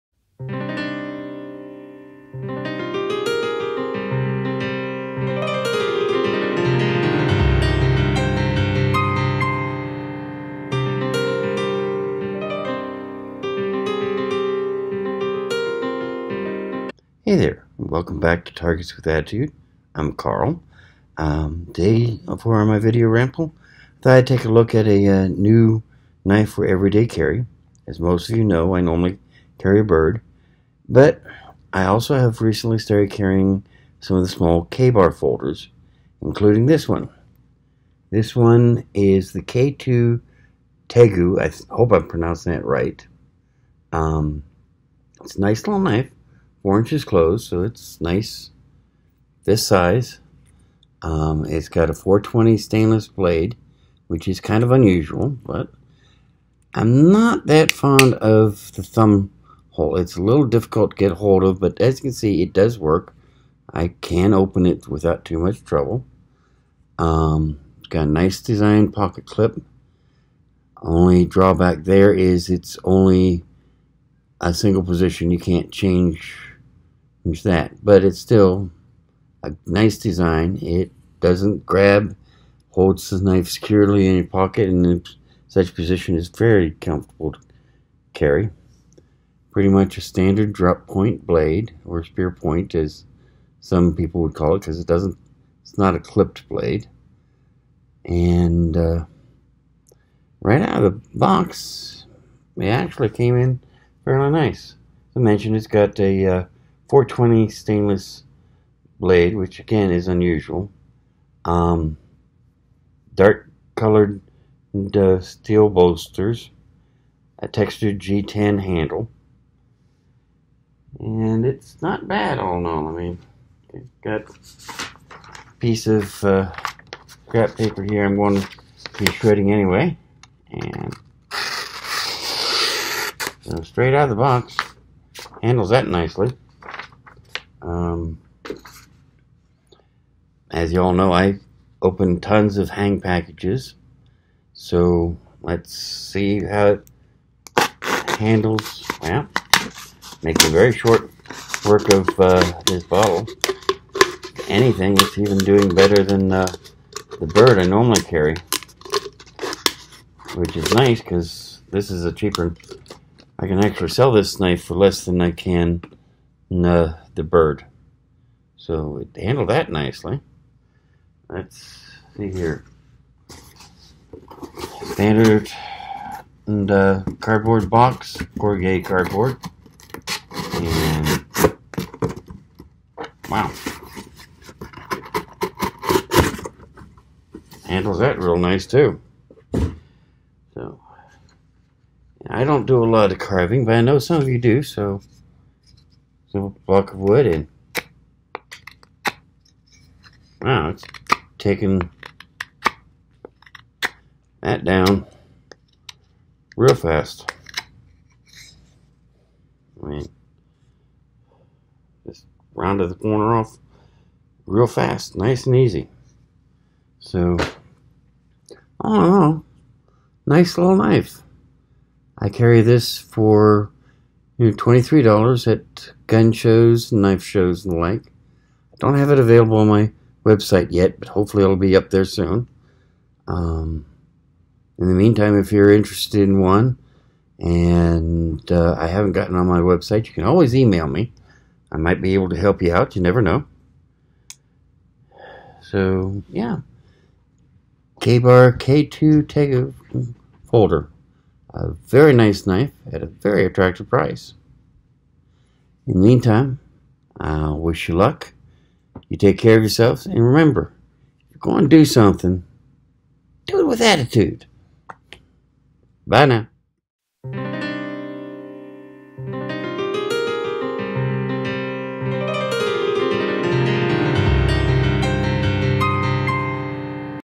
Ka Bar K2 Tegu
A new review/ramble on the Ka-bar K-2 Tegu folder.  This sweet little EDC knife passes our standard tests with flying colors, and best of all, it's a Ka-Bar!

If you like CRKT, check out : "The CRKT Fast Lane, New For 2019"